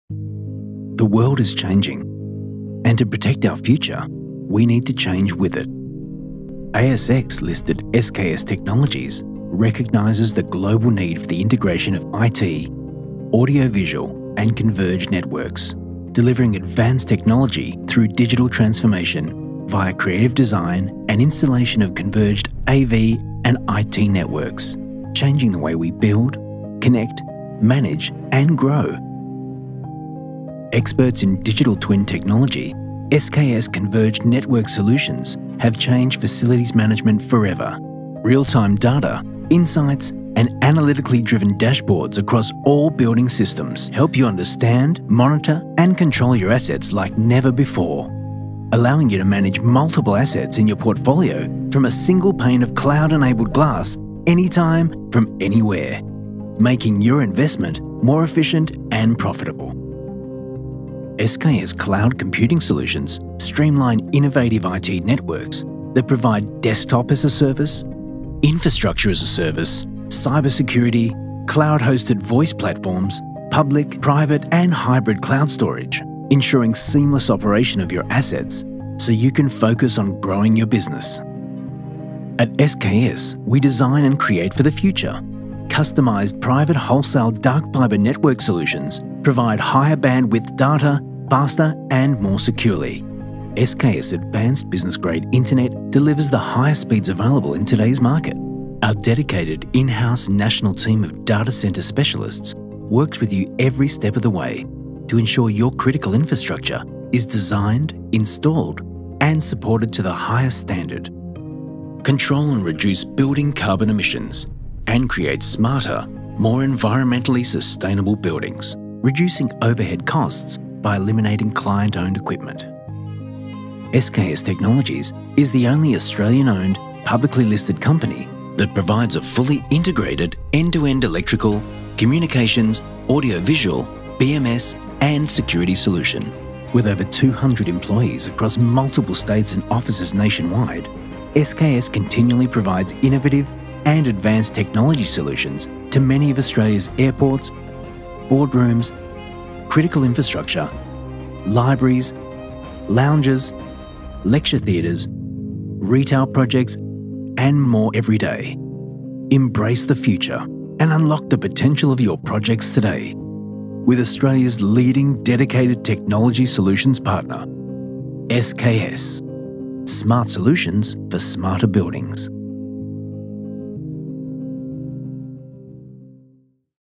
SKS Technologies Corporate Video
SKS Technologies (ASX: SKS) is excited to release our new short corporate video explaining who we are and our expanding capabilities.